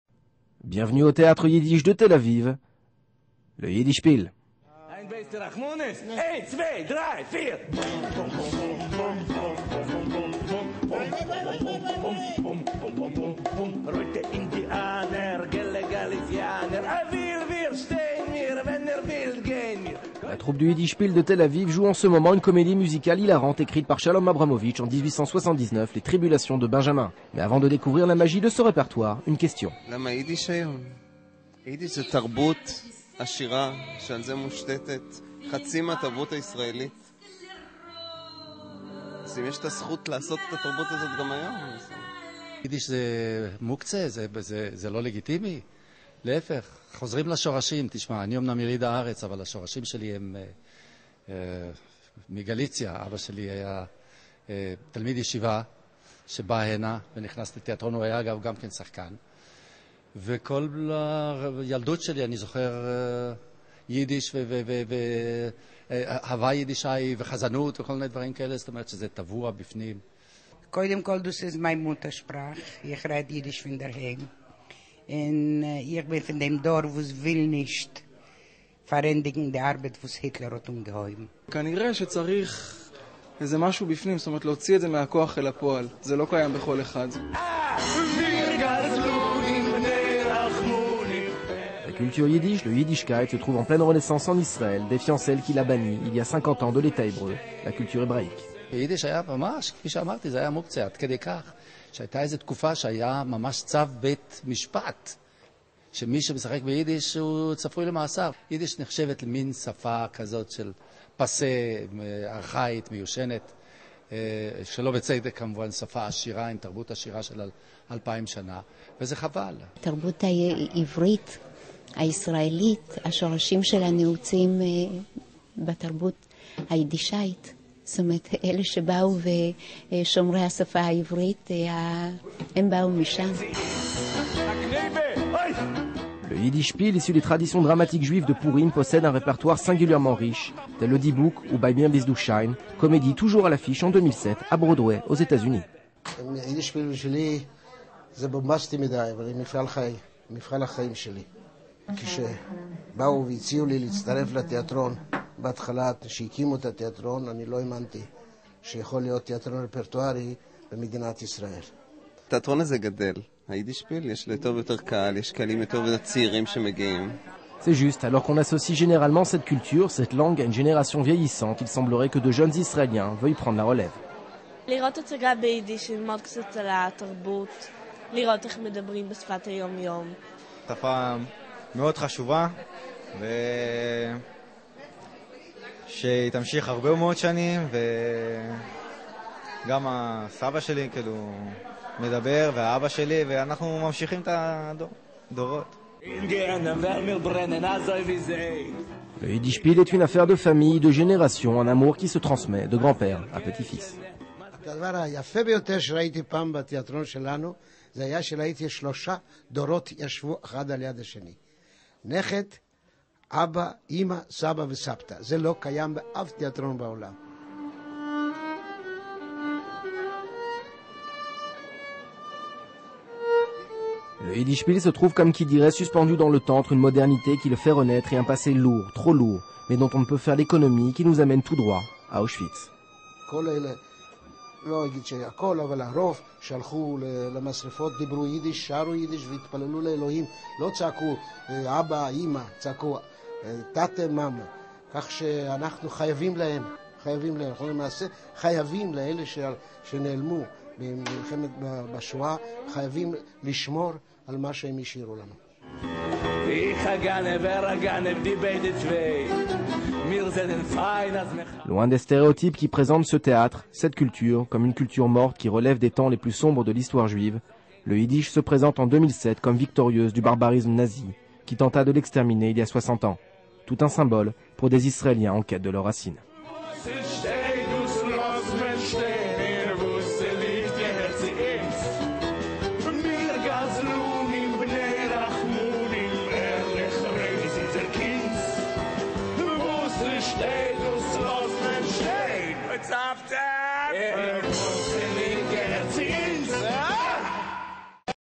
YIDDISH SPIEL A TEL AVIV
Découvrez la troupe du théâtre Yiddish de Tel Aviv qui donne en ce moment la pièce de Shalom Abramovitch, &quot;Les Tribulations de Benjamin&quot;. Perdue entre un humour décapant et une histoire tragique, la culture Yiddish, le Yiddishkeit se trouve en pleine renaissance au coeur d&#39;une société israélienne en quête de ses racines. Des images, des sonorités et une dérision sans nom, forment ce répertoire dramatique unique en son genre.